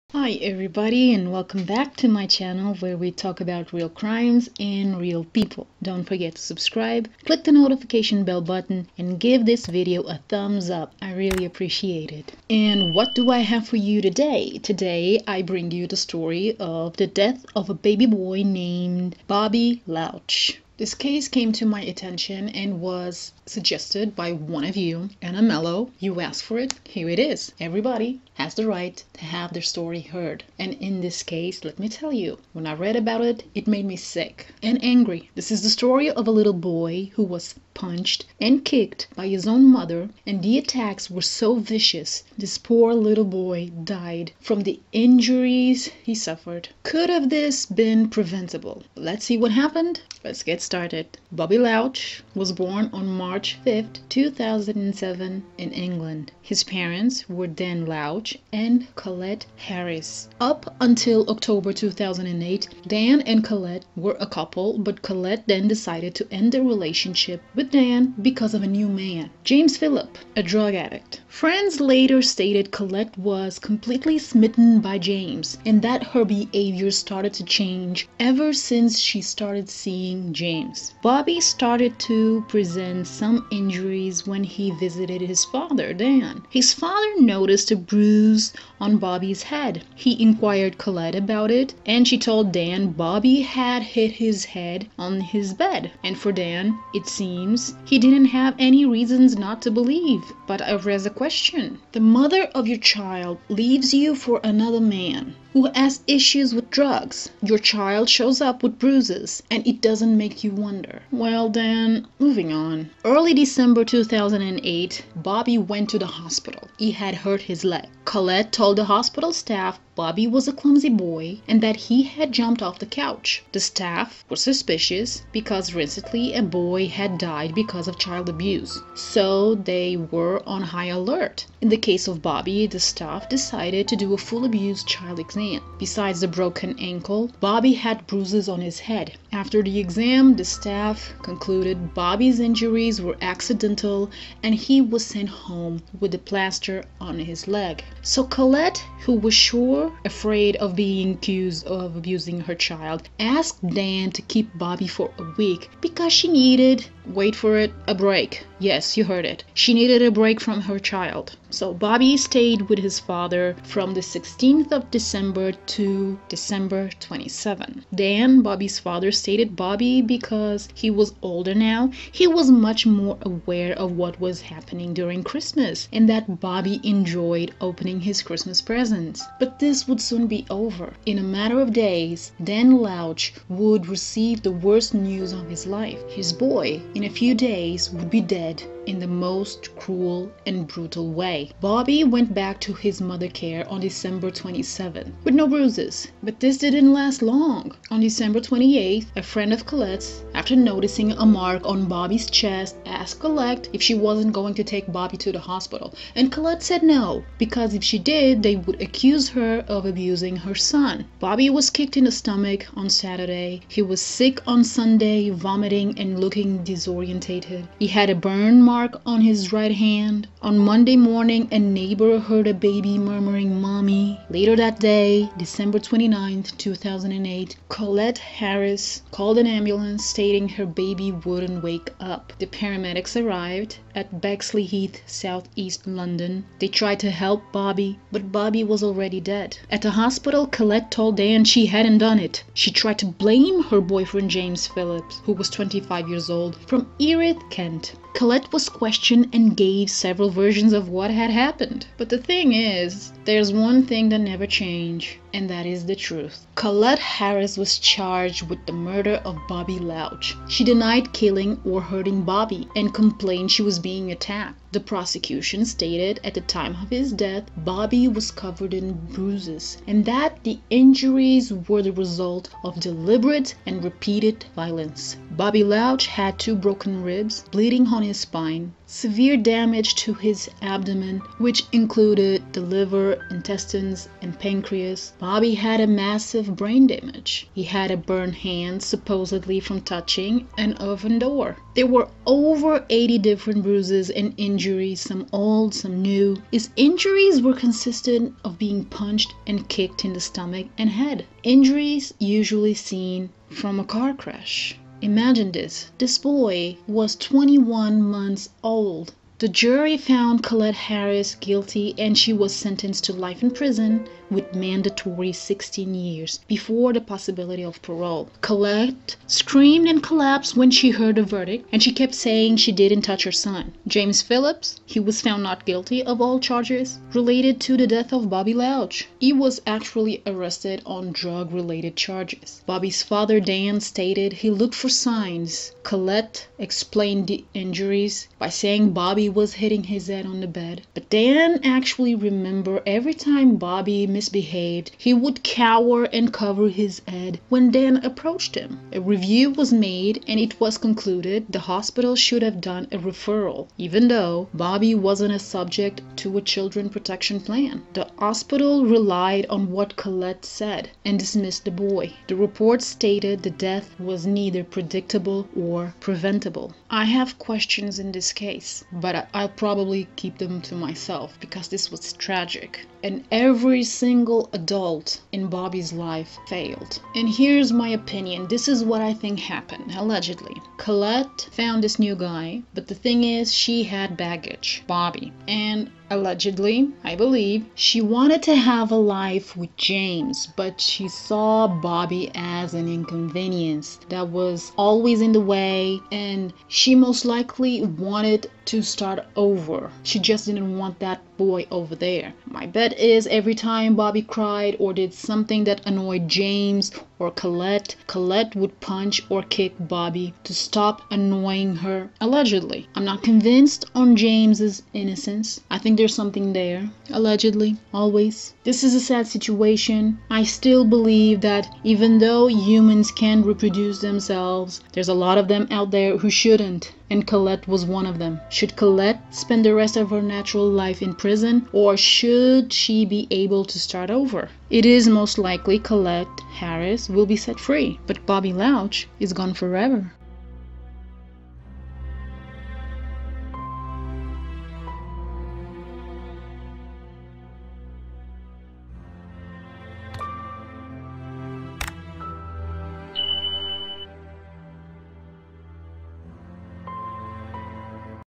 His mother did it.The violent and cruel death of Bobby Louch| True crime&Mystery
Today I bring the story of Bobby Louch, a 21 month old boy who had a violent short life.
Bobby's last days of his life were cruel.
The monster? His mother. The person who was supposed to protect Bobby was actually the one who hurt him in the most vicious way until Bobby couldn't take it anymore and met his demise.
Could his death been preventable?

0:00 Intro
0:51 Background
1:20 The signs
2:27 The last Christmas
2.59 Bobby Louch's slow and painful death
4:16 Trial,verdict and sentence
5:32 Aftermath
6:06 Final thoughts